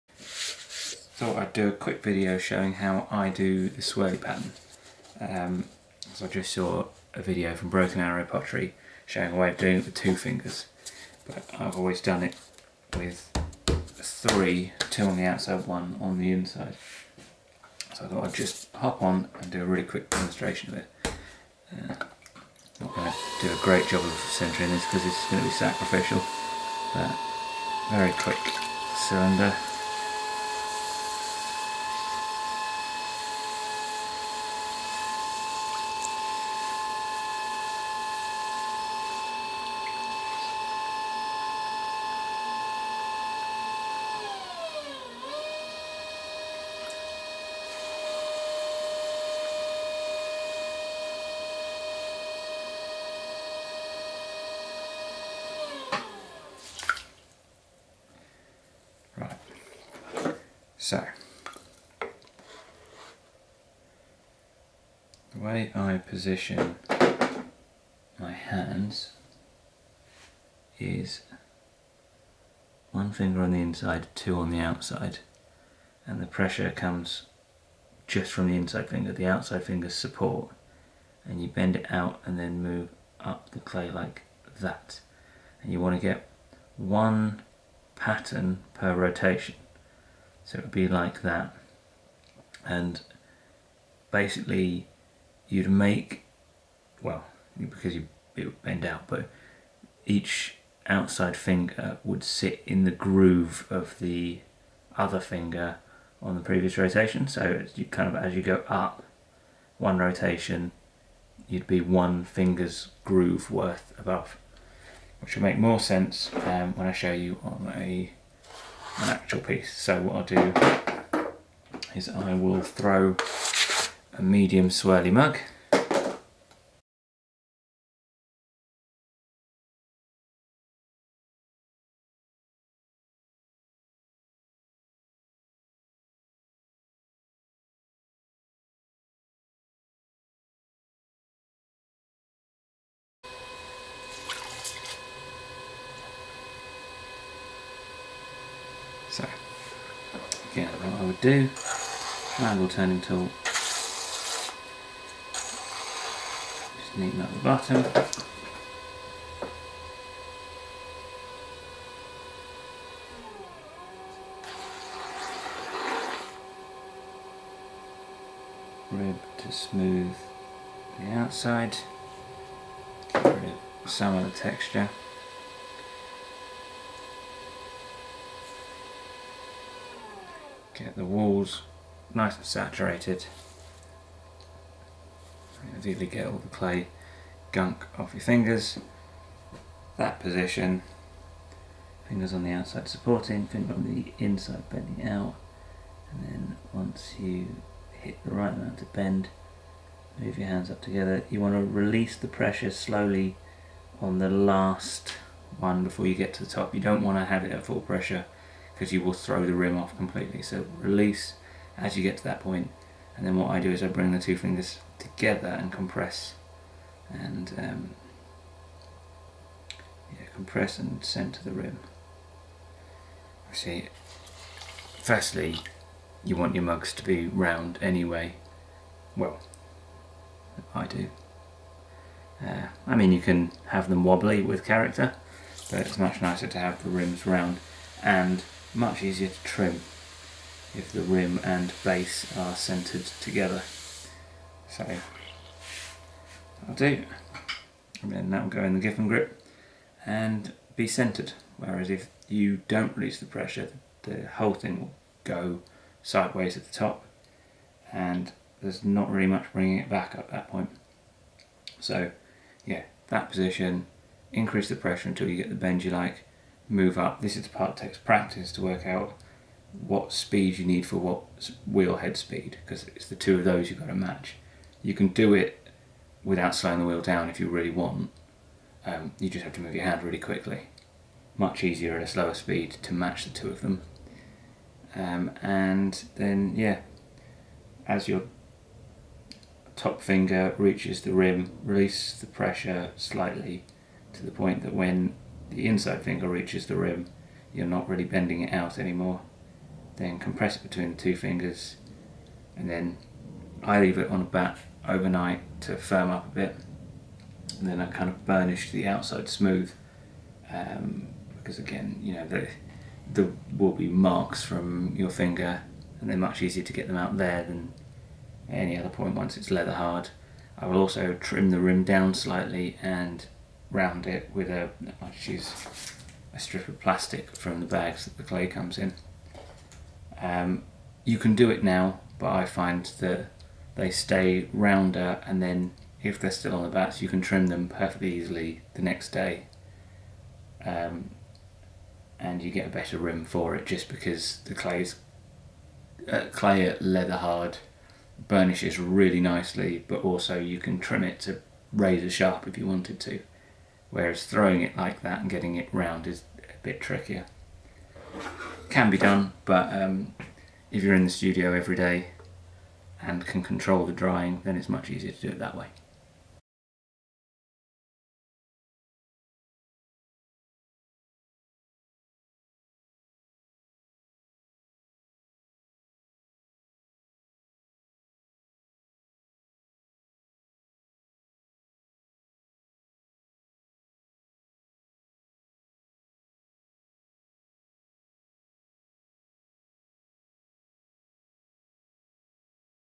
How to do the Swirly Pattern
The technique to add the Swirly pattern is incredibly simple, but there are a few tricks to make it easier. It's essentially an exaggerated pull, but moving up so fast relative to the wheel speed that after one rotation you're one Swirl higher.

The inside finger is the one that bends the clay, the two fingers outside support the clay above and below to give a clean bend to the clay. With my hands in that position, I gently increase the pressure from the inside finger until the clay starts to bend outwards, then both hands move up together.

If you get the movement speed right, the lower outside finger should be in the groove left by the upper outside finger on the previous rotation.

If you're having trouble with it, my main advice would be to:

Get the clay really wet first. Your fingers need to slide smoothly over the surface and the process only takes a few seconds, so you can really get the clay saturated.

Slow the wheel down. The slower you do it, the more control you have. It's possible to do it in one fast motion but it's much harder to correctly judge the speed or to correct it as you go.

Practice on smaller/thinner pieces. The thicker the walls of a piece, the harder it is to bend the clay. This makes every part of the process trickier too.You can't easily trim the walls after you've done this, so you want to throw to the final thickness.

I use my fingertips to burnish the outer walls when the clay is leather hard. I also use a strip of flexible plastic to burnish the rim at the same time. I leave them on the batt until then as it makes this stage easier, then wire them off after. At this point they should be solid enough to easily remove them from the batt without deforming or marking them.